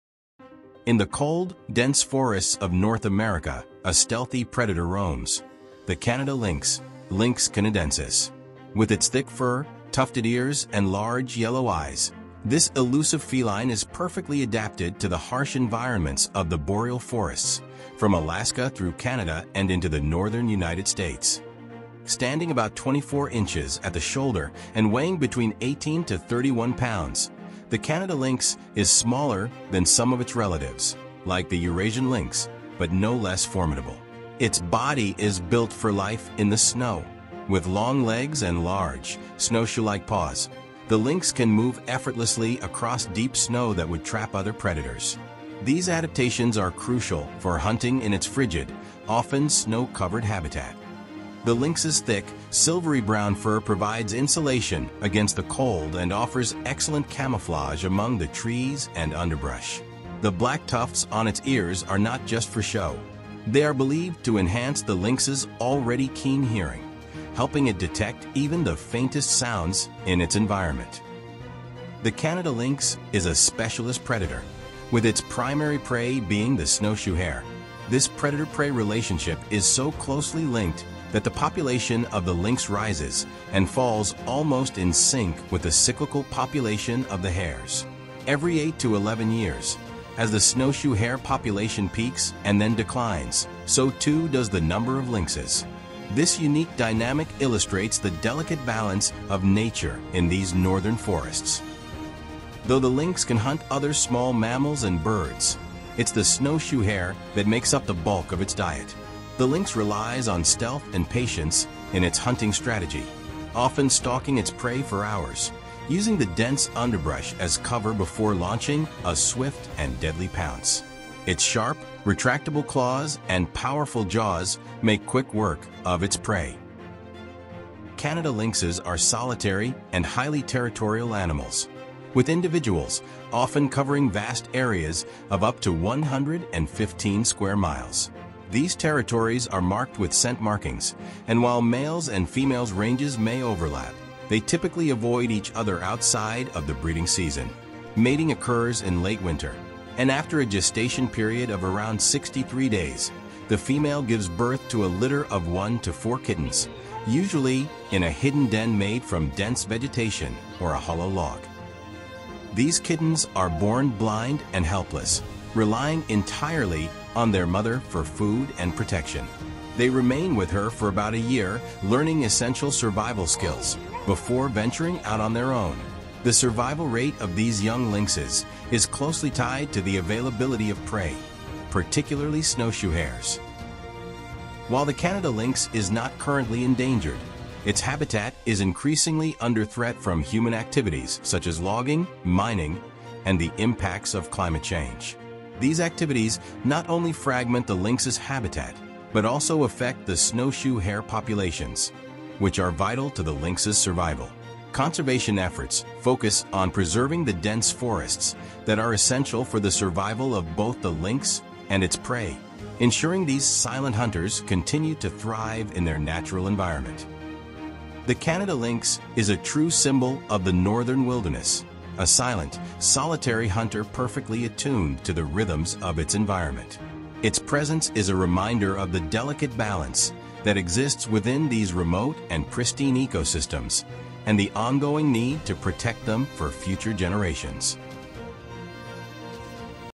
Canada Lynx || The Arctic Cat of North America
Explore the fascinating world of the Canada lynx, a silent and solitary hunter perfectly adapted to the harsh environments of the boreal forests. In this video, we dive deep into the life of this elusive feline, known for its thick fur, tufted ears, and large snowshoe-like paws that allow it to effortlessly navigate the snowy landscapes of North America.

Discover how the Canada lynx’s survival is intricately linked to its primary prey, the snowshoe hare, and learn about the unique predator-prey relationship that drives the population dynamics of this remarkable species. We’ll also take you through the lynx’s hunting techniques, breeding habits, and the challenges it faces in a rapidly changing world.

With stunning visuals and detailed insights, this video sheds light on the Canada lynx’s role as a key predator in its ecosystem and the adaptations that make it such an effective hunter. Whether you’re a wildlife enthusiast or simply curious about this mysterious cat, join us on a journey into the heart of the northern wilderness to uncover the secrets of the Canada lynx.

00:00 - Intro
00:05 - Canada lynx
00:21 - Characteristics
01:11 - Diet and hunting
02:07 - Home ranges
02:27 - Reproduction
03:03 - Conservation